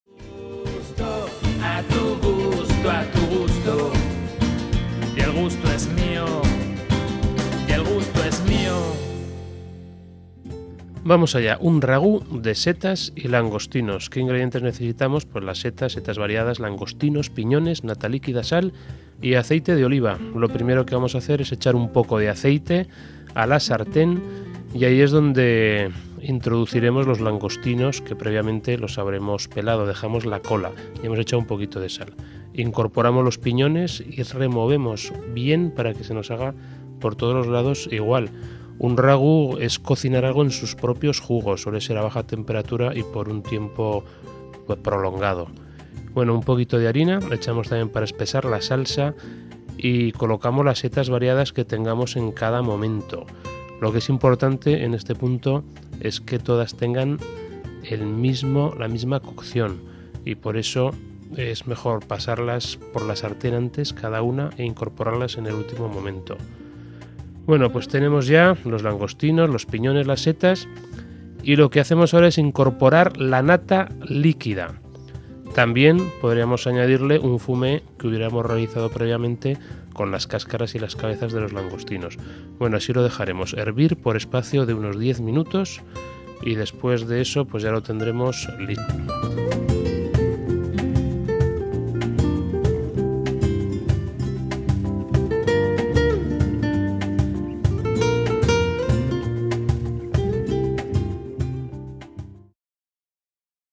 Ragú de setas con langostinos
Saltearemos unos langostinos con piñones, le añadiremos las setas variadas, nata líquida y un fumet hecho con las cabezas y cáscaras de los langostinos